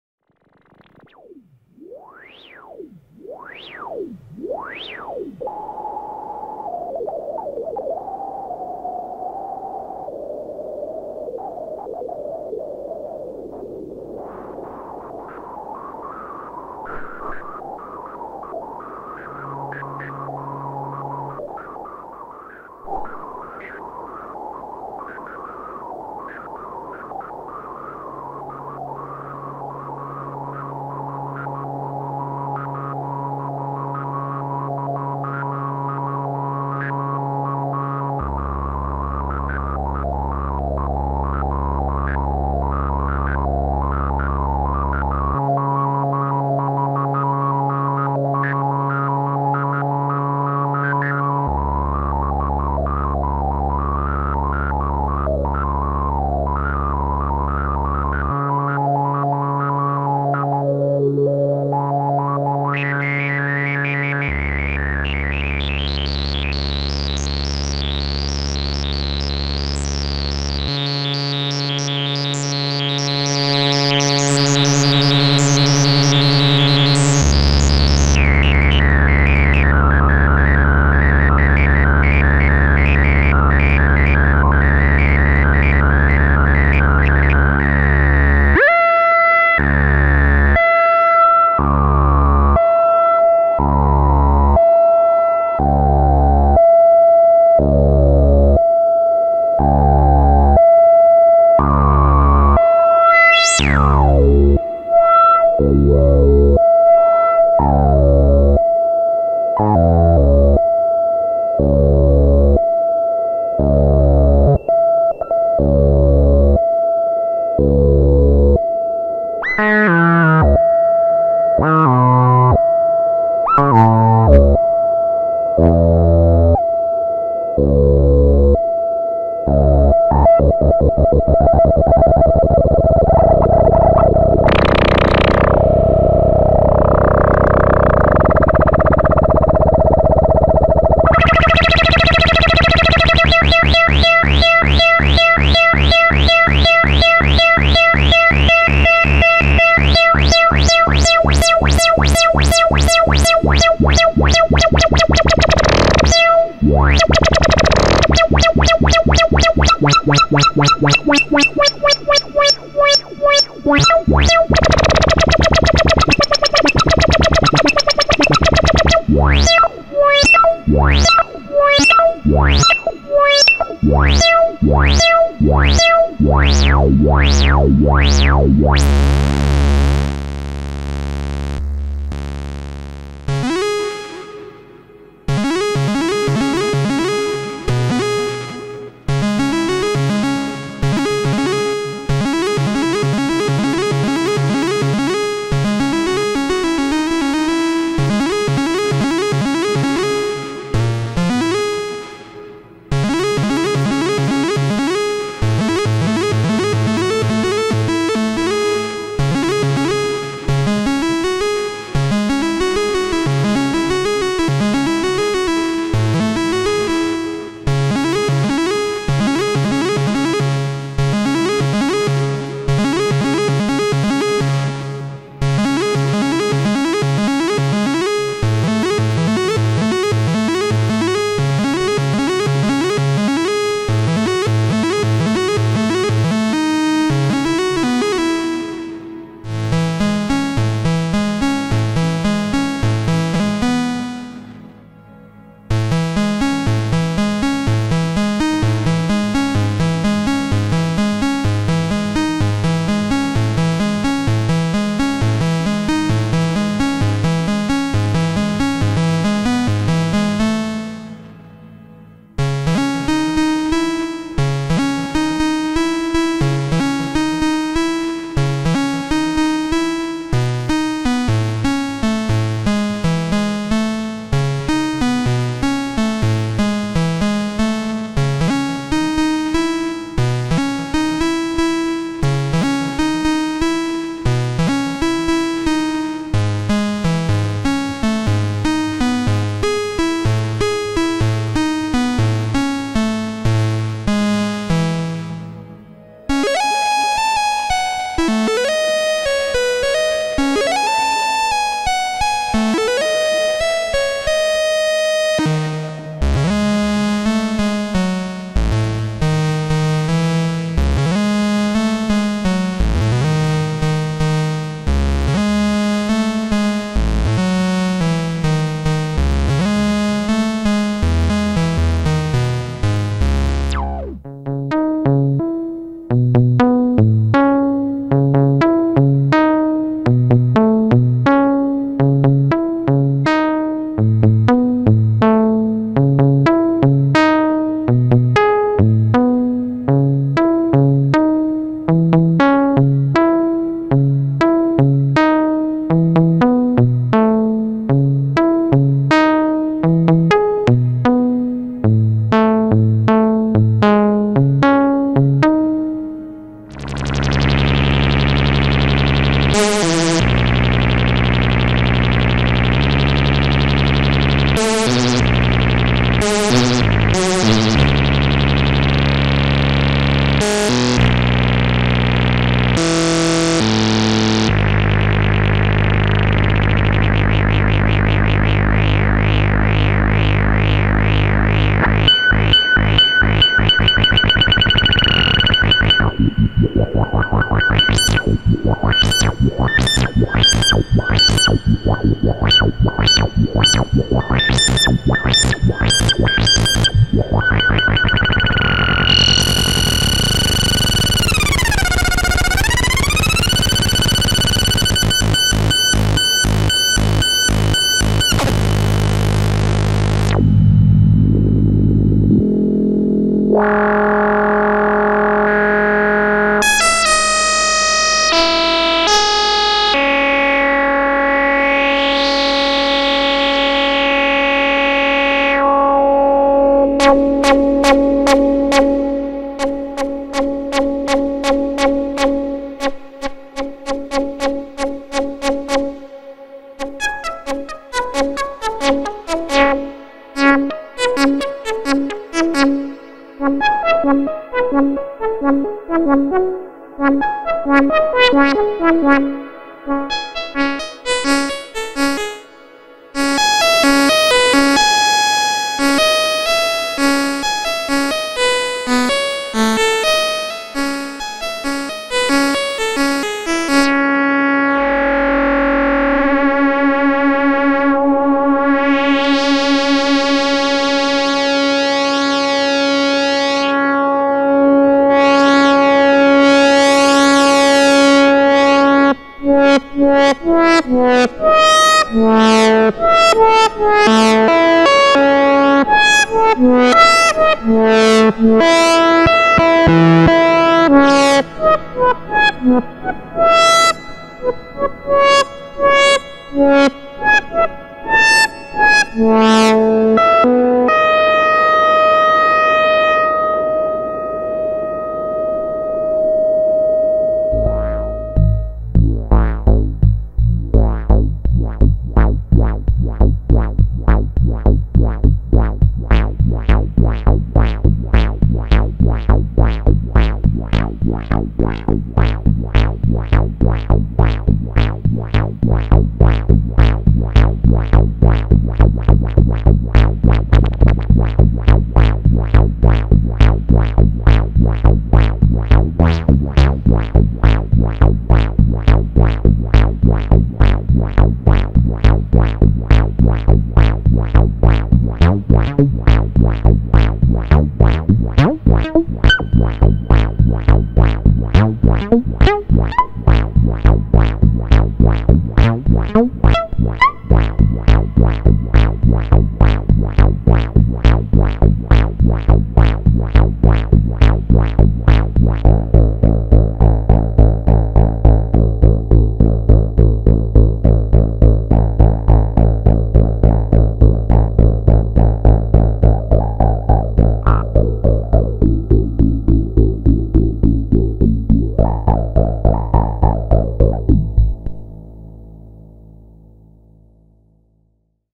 Ритм - 2, неудачные дубли, невошедшие в выпуск.
При записи больших обзоров часто остается много материала, который по разным  причинам (ошибки при игре, незаконченные отрывки,  дубли, неудачное кадрирование) не попадает в основной выпуск трепологии. Выкладываю нарезку из таких дублей.